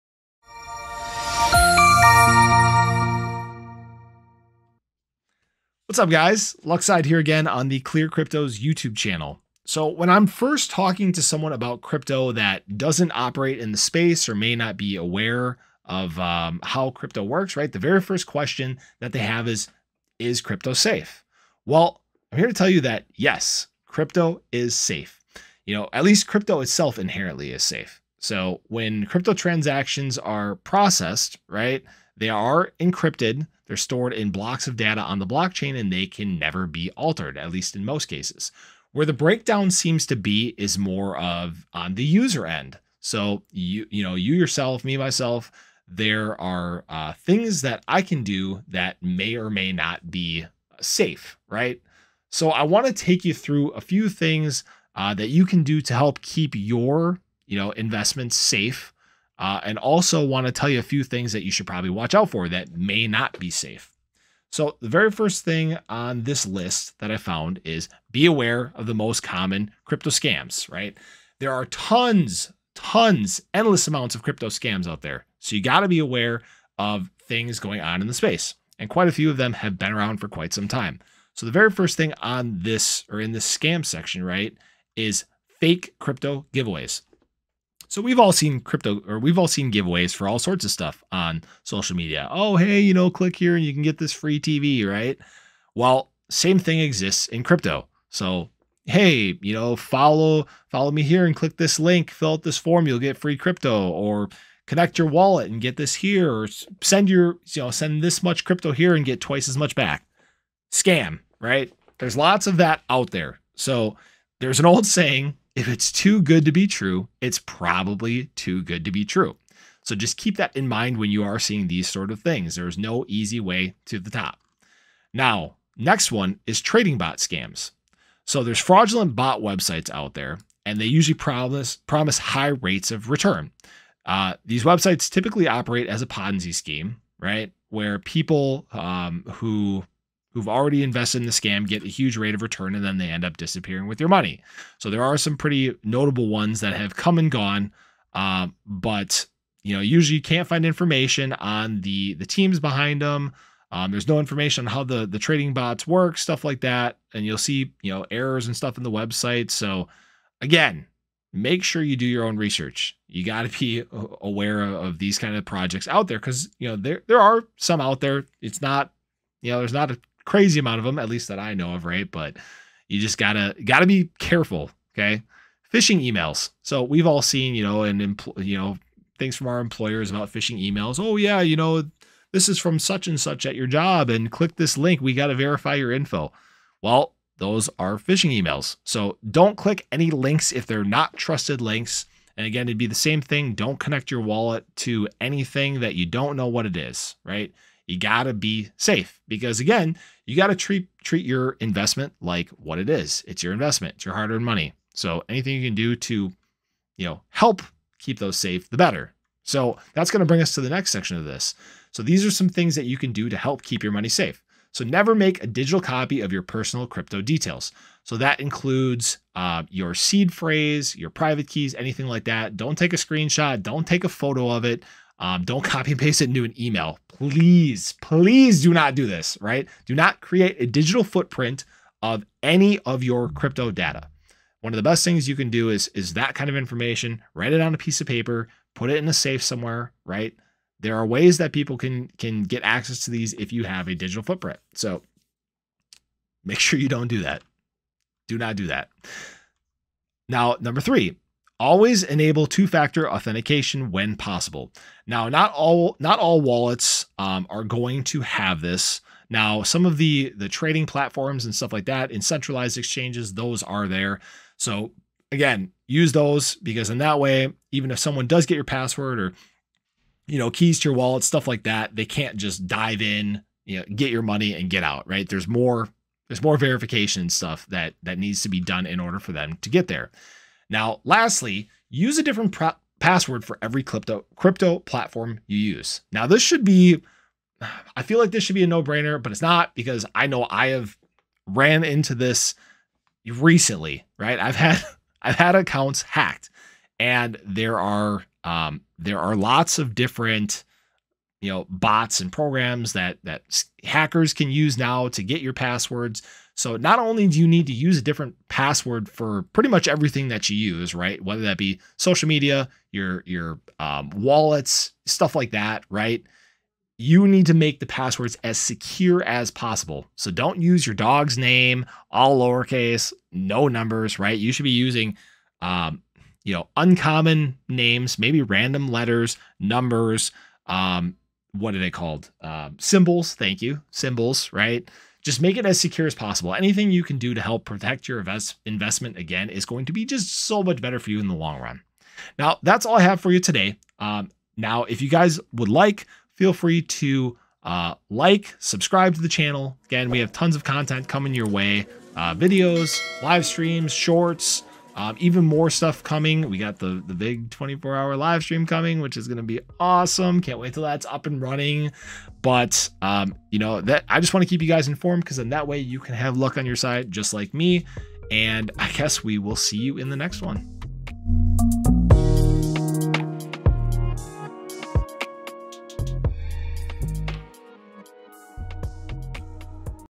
Is Crypto Safe - How Can I Protect Myself?
Description - Many people wonder if crypto is in fact safe.  There are many ways to ensure you are operating safely in the space and we cover a few of them in this video.